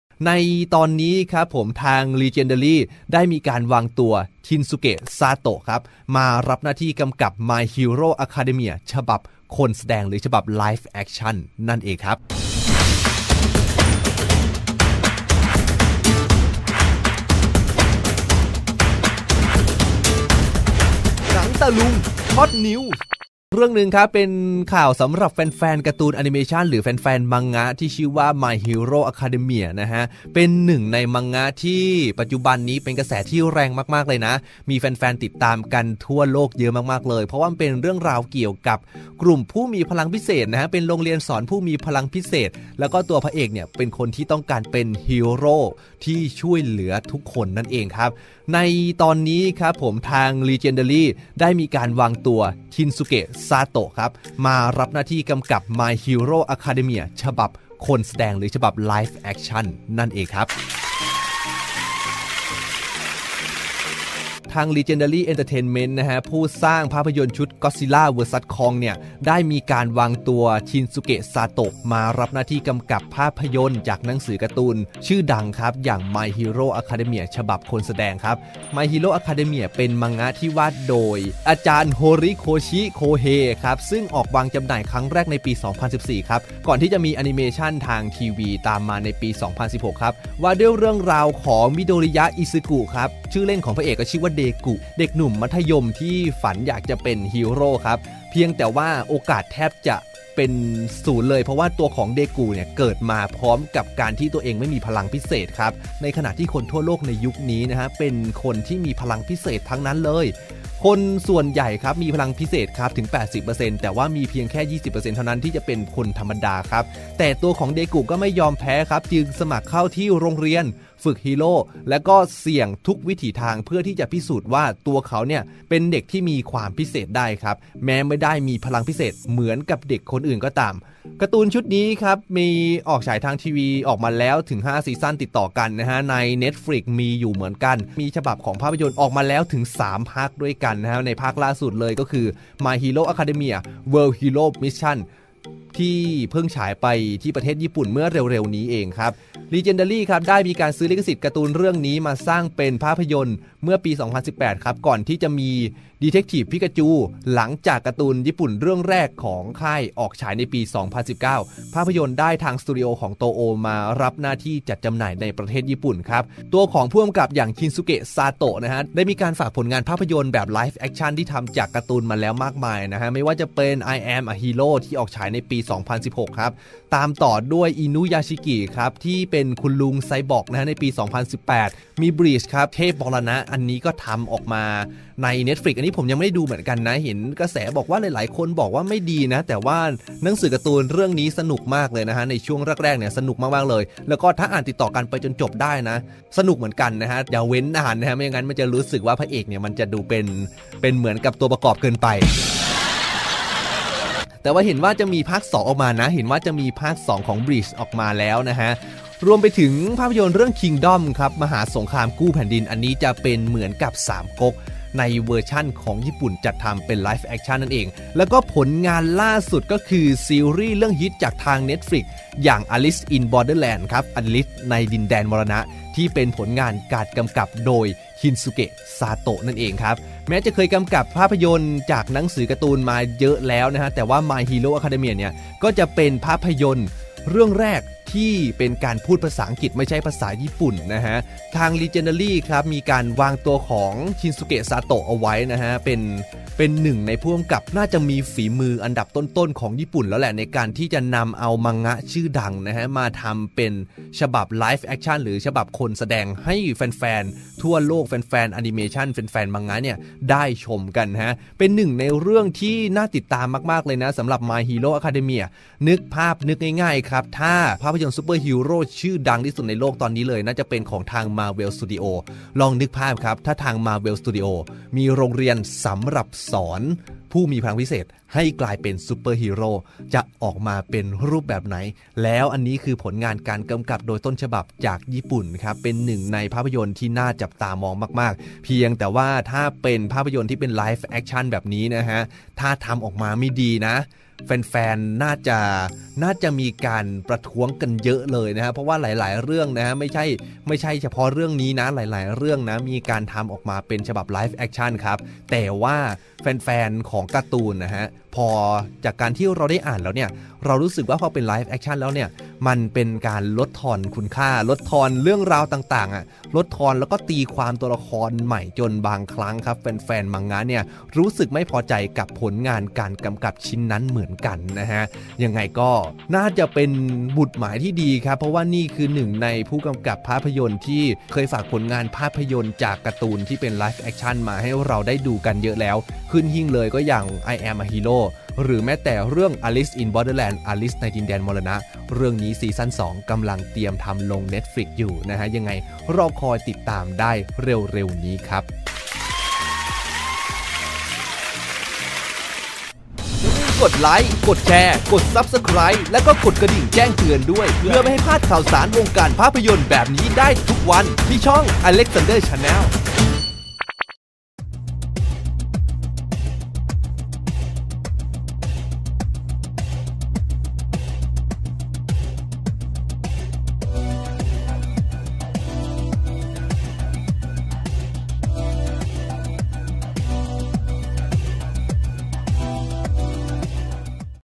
My Hero Academia ฉบับ Live Action ได้ชินสุเกะ ชาโตะ มากำกับ
My Hero Academia Live Action ได้ชินสุเกะ ชาโตะ มากำกับ "หนัง-ตา-ลุง SHOT NEWS"
จากการ์ตูนชื่อดัง สู่ภาพยนตร์ฟอร์มยักษ์ My Hero Academia Live Action

ติดตามความเคลื่อนไหวของรายการ